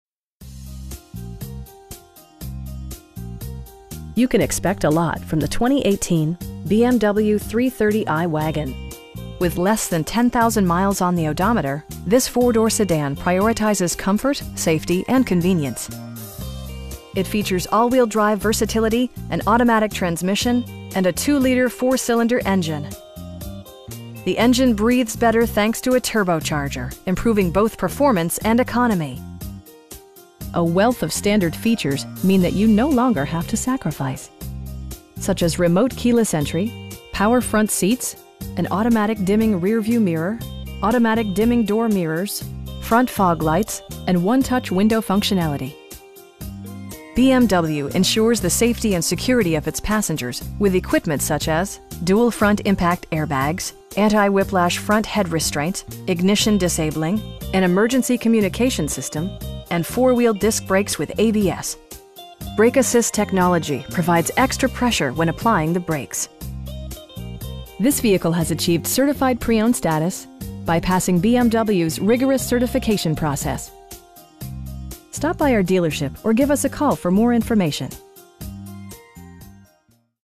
2018 BMW 330i xDrive in Fletcher, NC 28732
You can expect a lot from the 2018 BMW 330i wagon With less than 10,000 miles on the odometer, this 4 door sedan prioritizes comfort, safety and convenience. It features all-wheel drive versatility, an automatic transmission, and a 2 liter 4 cylinder engine. The engine breathes better thanks to a turbocharger, improving both performance and economy. A wealth of standard features means that you no longer have to sacrifice. Such as remote keyless entry, power front seats, an automatic dimming rear-view mirror, an outside temperature display, automatic dimming door mirrors, front bucket seats, front fog lights, and 1-touch window functionality. BMW ensures the safety and security of its passengers with equipment such as: dual front impact airbags, front side impact airbags, traction control, anti-whiplash front head restraints, ignition disabling, an emergency communication system, and 4 wheel disc brakes with ABS. Brake assist technology provides extra pressure when applying the brakes. This vehicle has achieved Certified Pre-Owned status, by passing BMW's rigorous certification process. We pride ourselves in the quality that we offer on all of our vehicles. Stop by our dealership or give us a call for more information.